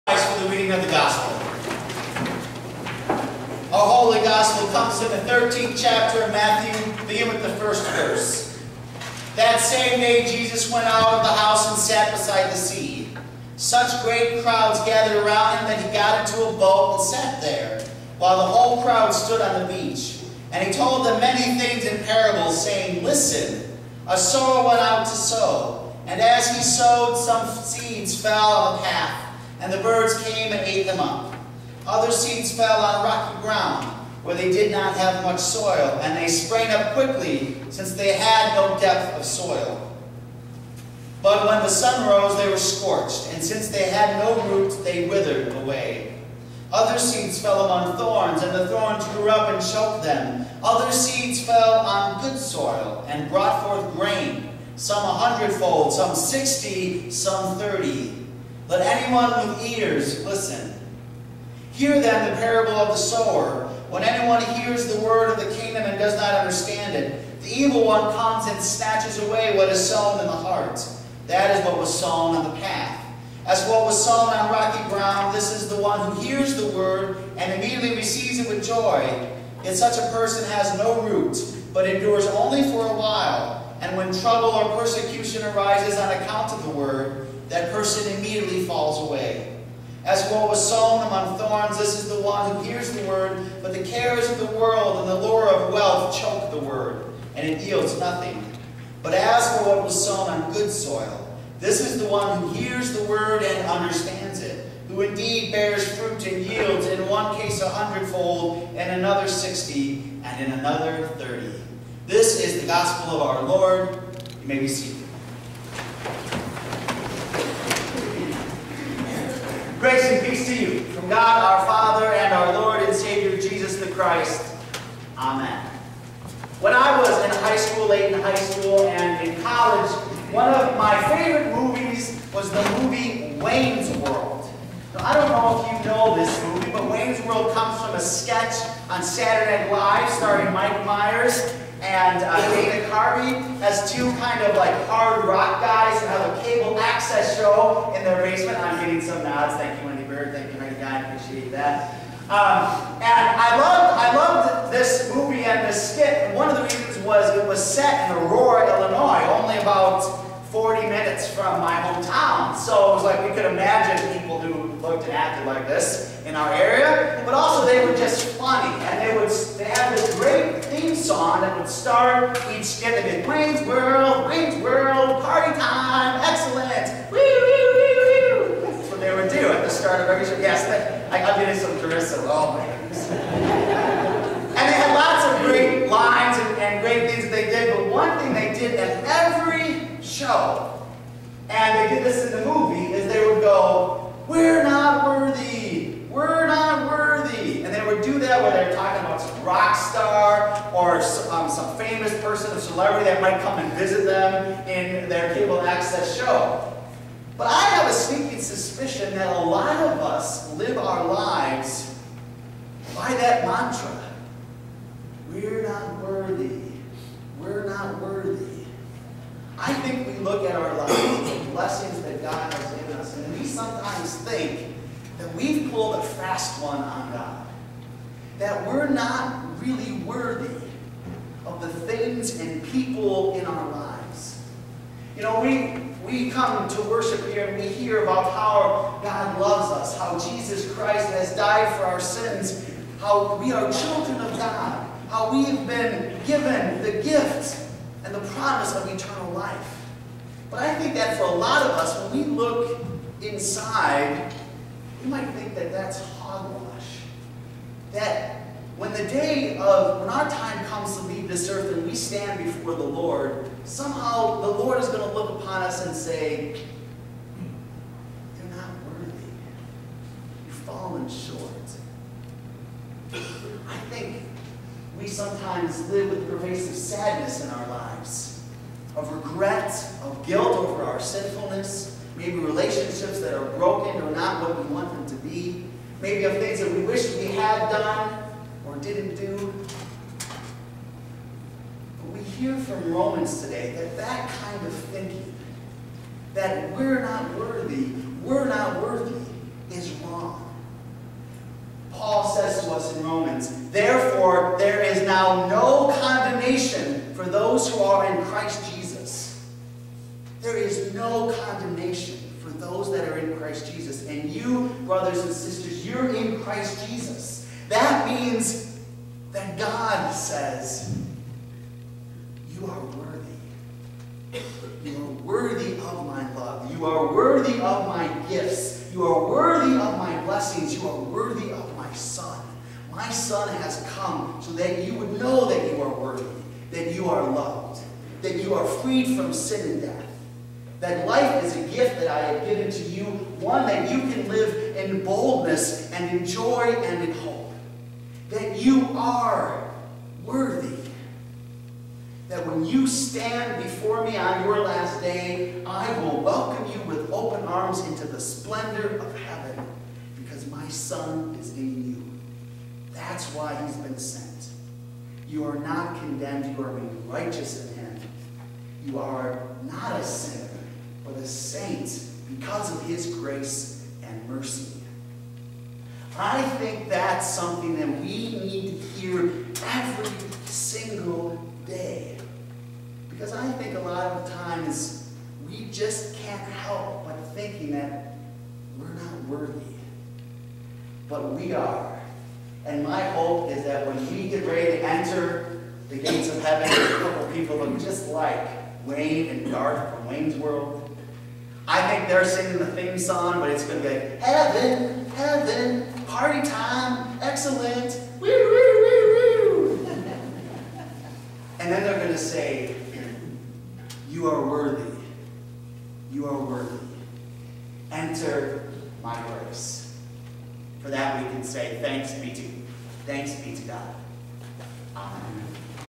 Gospel and Sermon July 12 2011.mpg
Gospel reading and sermon from July 12, 2011, at Trinity Lutheran Church, Boyceville, WI.  Preacher is Pastor Brad Peterson.  Sermon title is, "You are worthy."